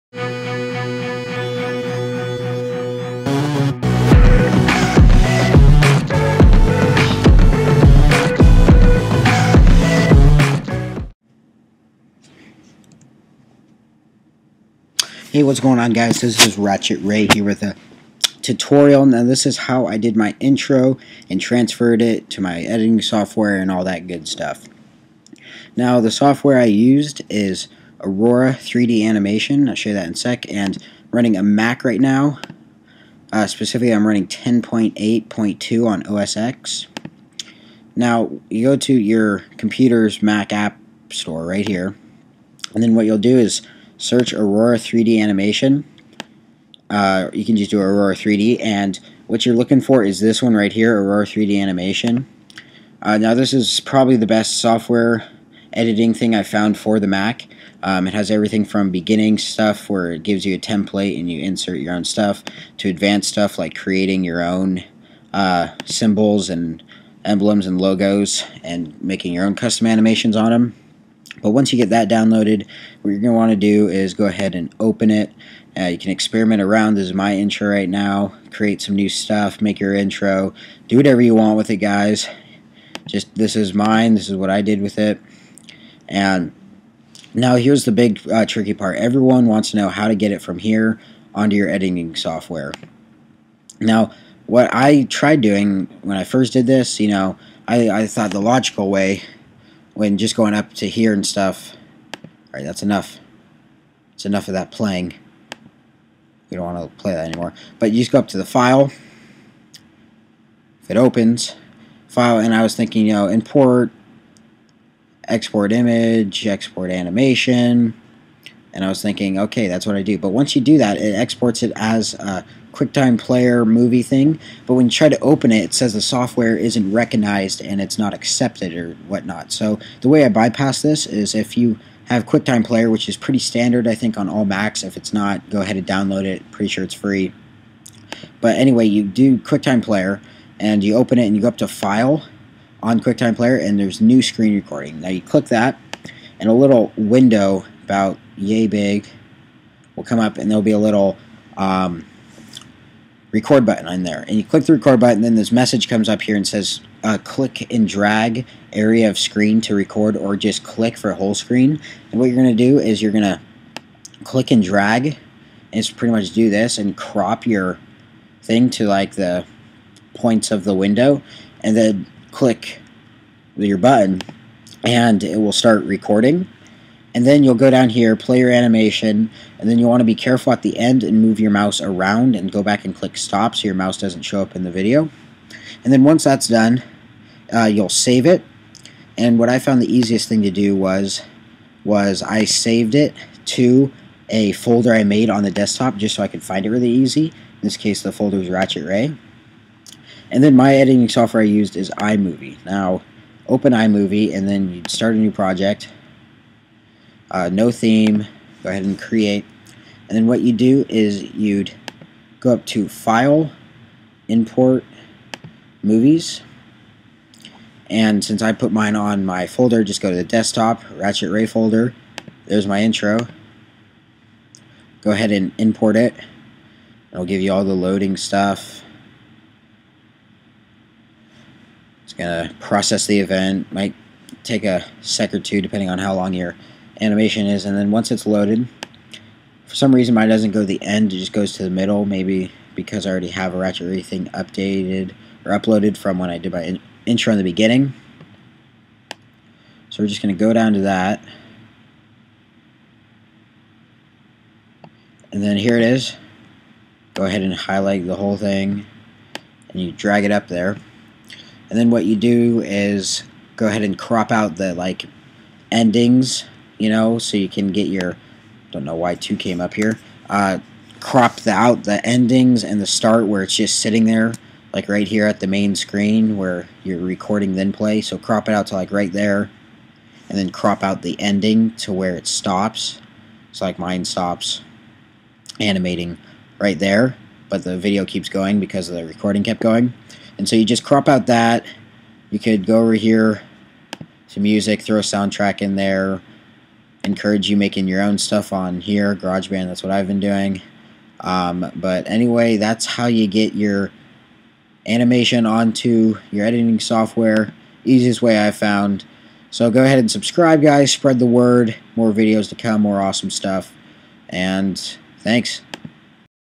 Aurora 3D Animation Help | How to Transfer intro to imovie
Hey Guys This is the best way i have found to transfer your intros to imovie or what ever software editor your using. let me know if it works and any other questions you have i always respond.